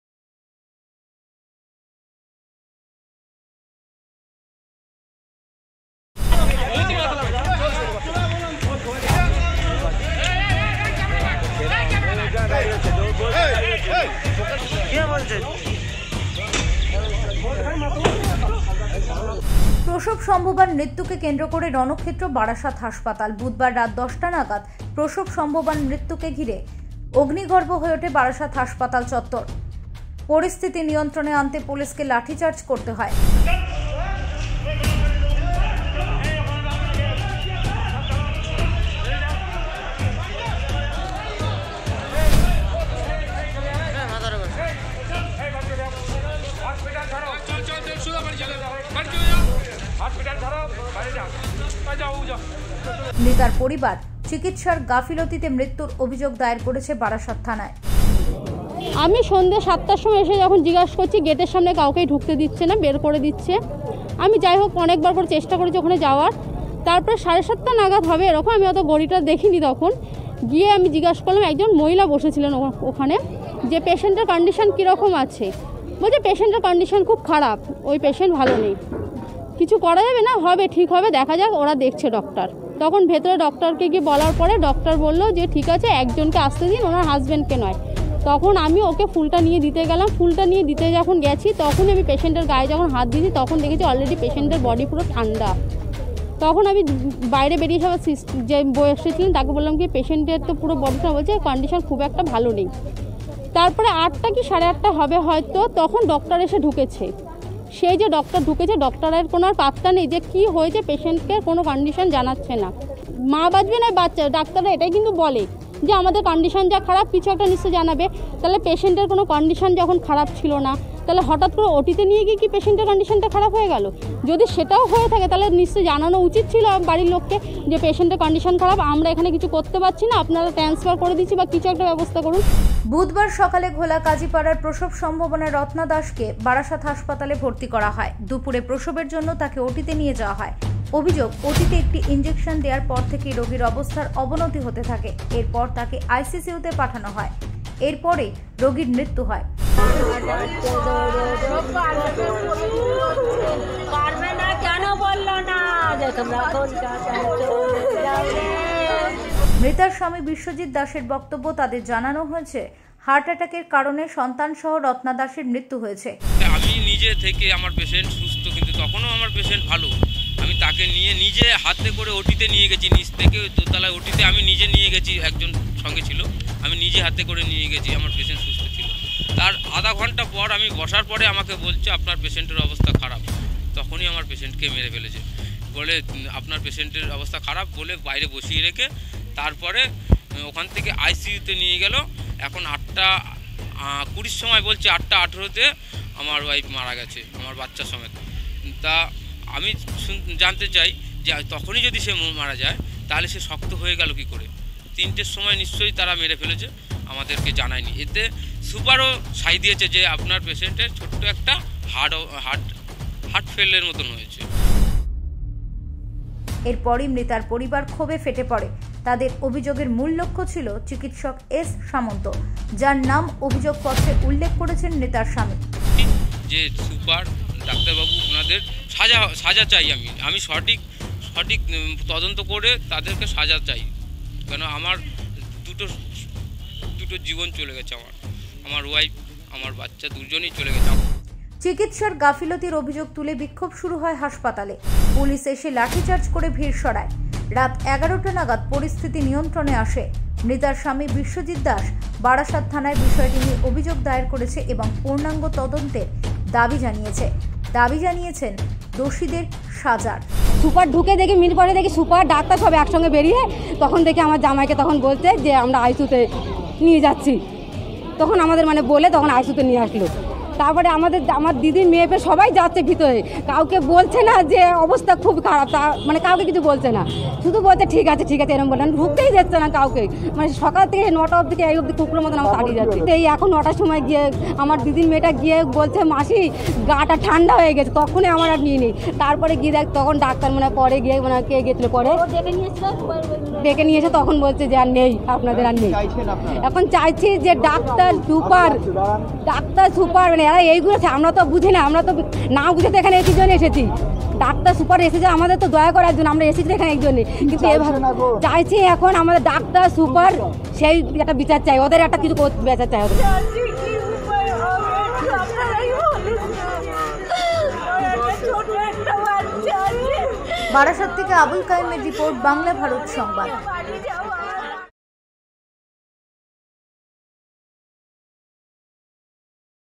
প্রসবসম্ভবার মৃত্যু ঘিরে অগ্নিগর্ভ বারাসাত হাসপাতাল#banglabharat#bbharat#bbharatsambad#barasat
প্রসূতির মৃত্যুর গাফিলতির অভিযোগ, উত্তপ্ত Barasat হাসপাতাল| মৃতের পরিবারের অভিযোগ ভুল চিকিৎসার জন্য এই অবস্থা হয়েছে